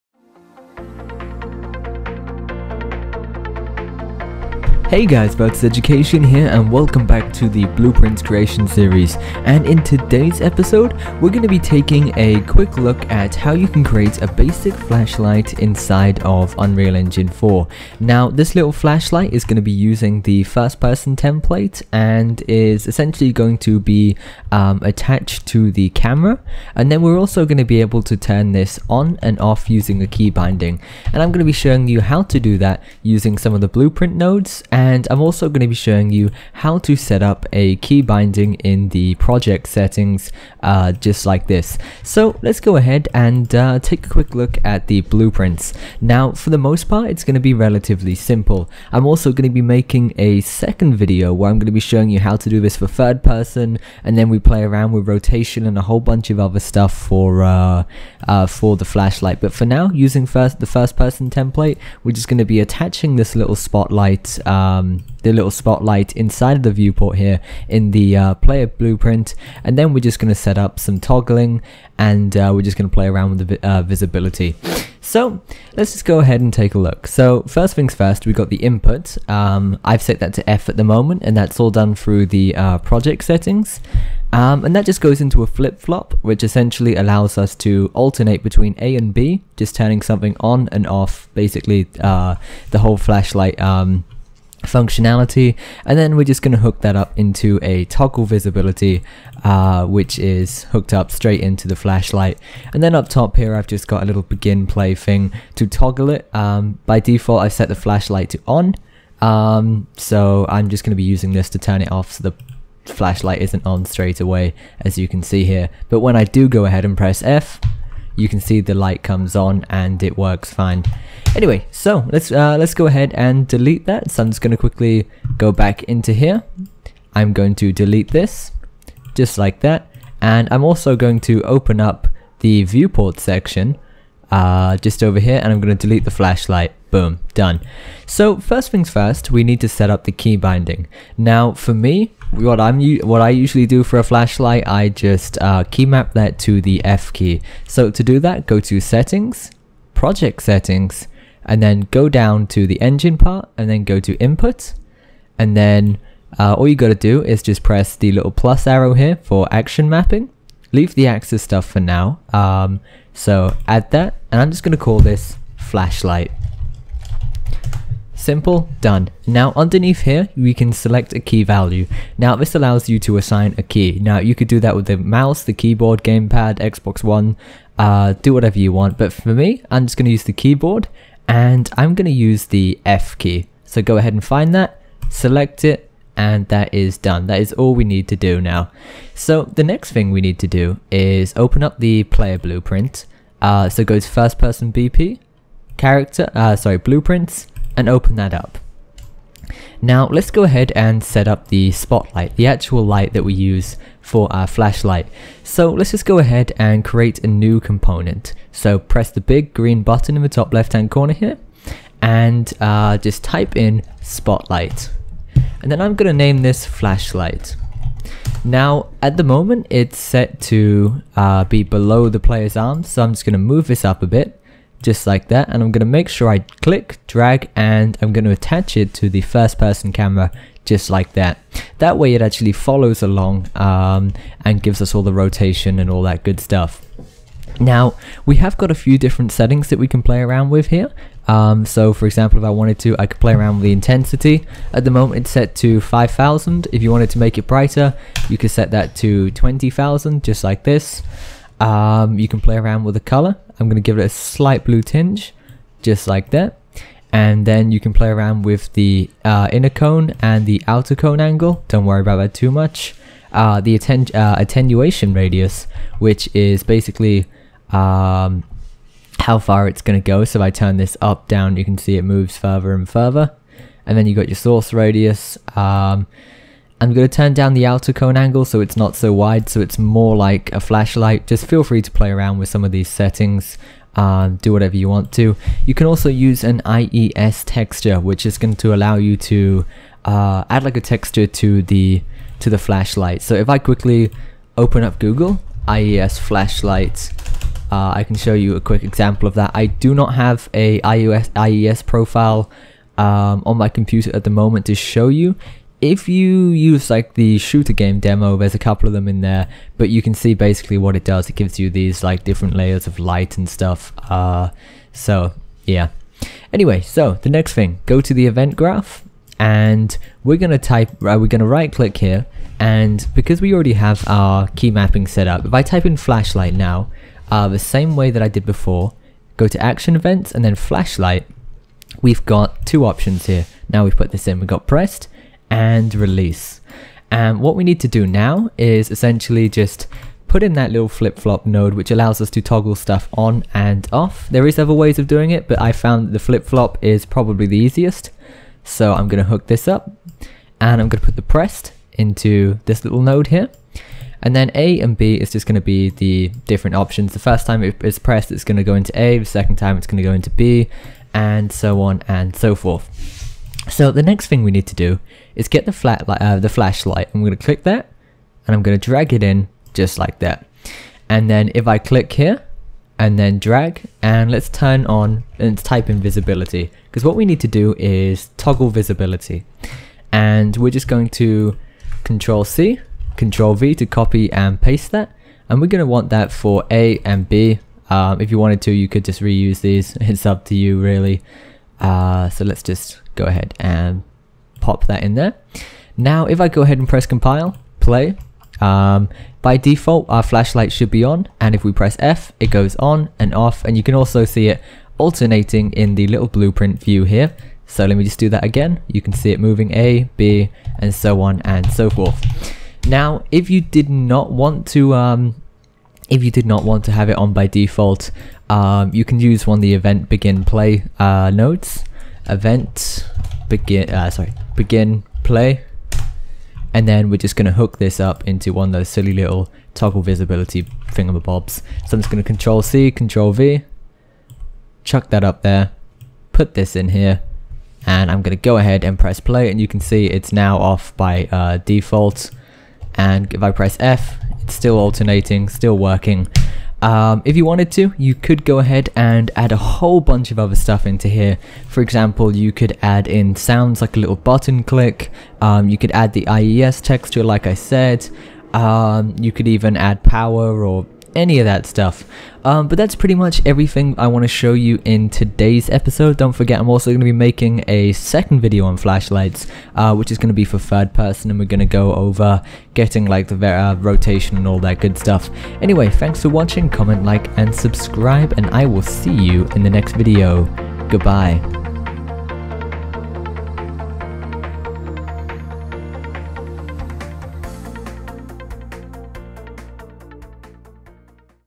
Unreal Engine 4 Blueprint Creations - #3 Simple Flashlight
This is episode 3 of my Unreal Engine 4 Blueprint Creations Series where we create a variety of scripts inside of blueprints to get a solid understanding of how blueprints work and how we can create mechanics and assets to form our own game.

In this video we show you how to create a basic flashlight using the player blueprint, adding a spotlight, attaching it and then setting up a keybinding for it in the project settings.

Next Video
In the next video we'll be creating another flashlight for use in third person, covering correct rotation and a few extra bits and bobs.